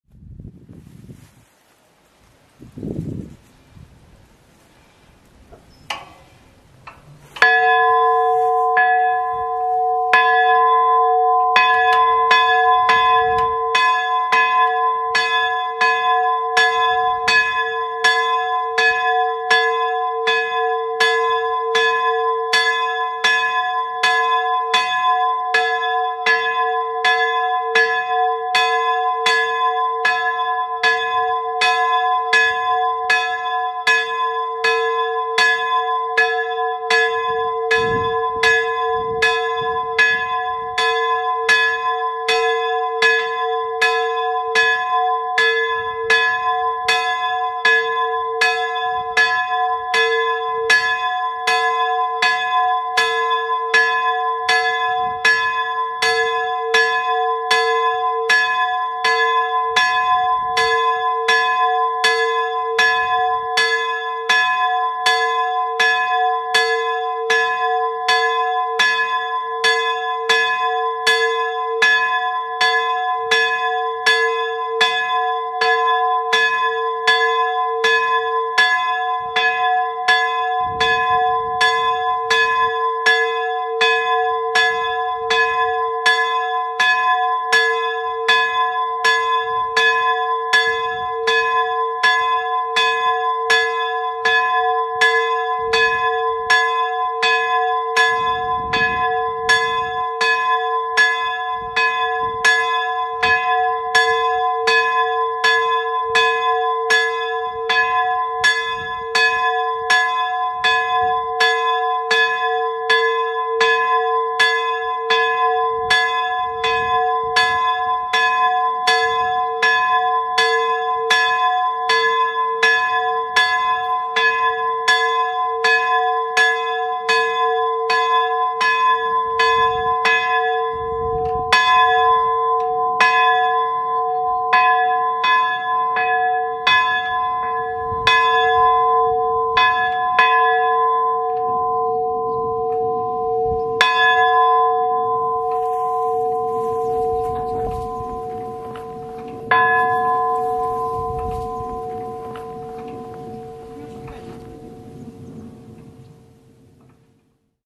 Świniary Wielkie - Dzwon kościoła pw. św. Bartłomieja
Na dzwonnicy obok kościoła zawieszony jest pochodzący z 1622 roku dzwon, który został ufundowany przez Adama Posadowskiego, ówczesnego właściciela Wołczyna i Świniar

Dzwon:
Waga: ok. 80 kg
Ton: a" (-)

Opis wizualny pojawi się wkrótce